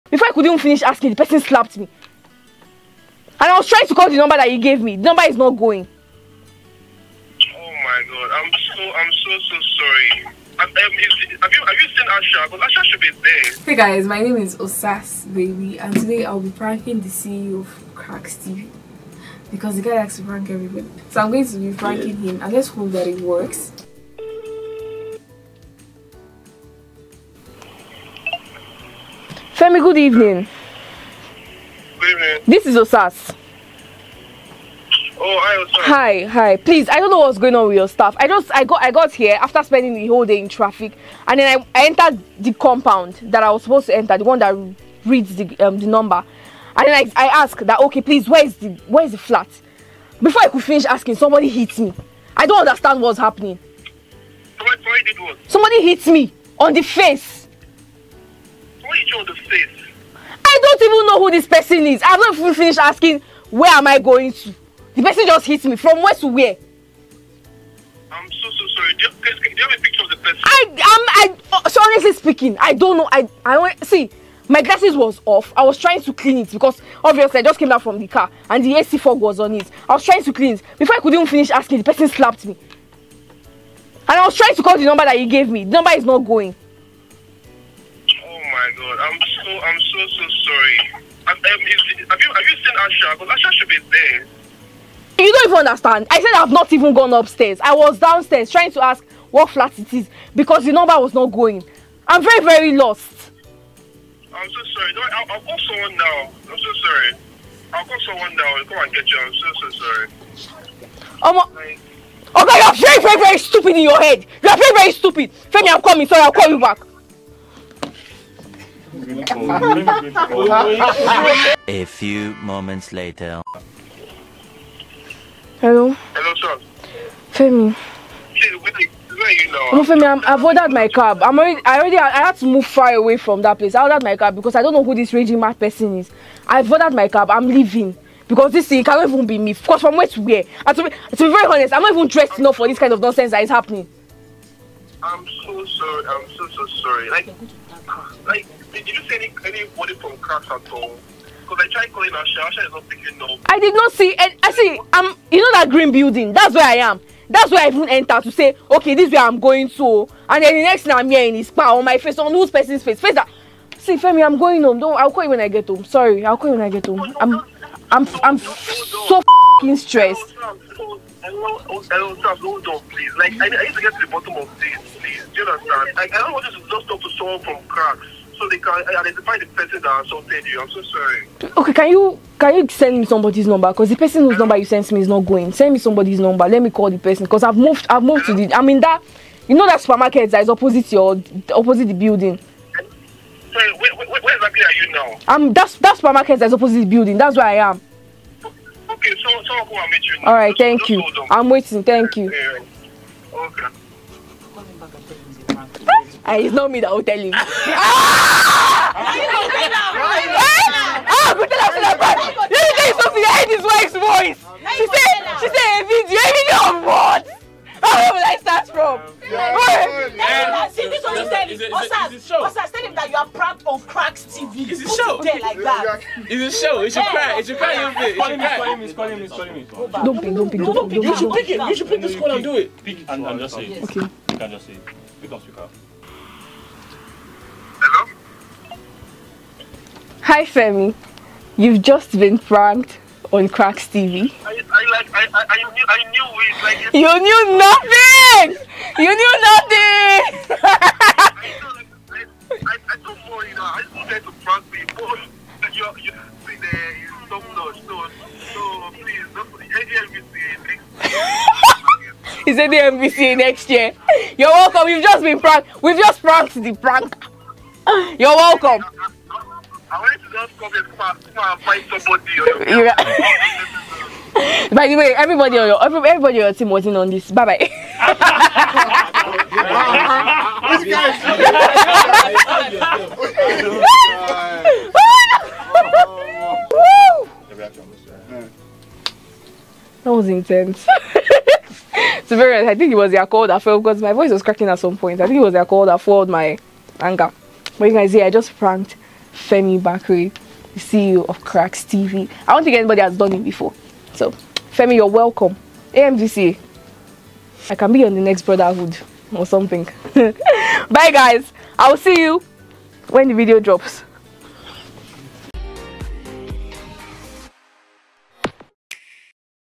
OSAS Pranks FEMI BAKRE (CEO of Kraks Media) | Celebrity Pranks
OSAS was asked to prank a celebrity, her choice was FEMI BAKRE, the CEO of Kraks Media.

Watch this hilarious prank call between Osas and Femi Bakre and find out if he fell for the prank or not.

for the most hilarious content!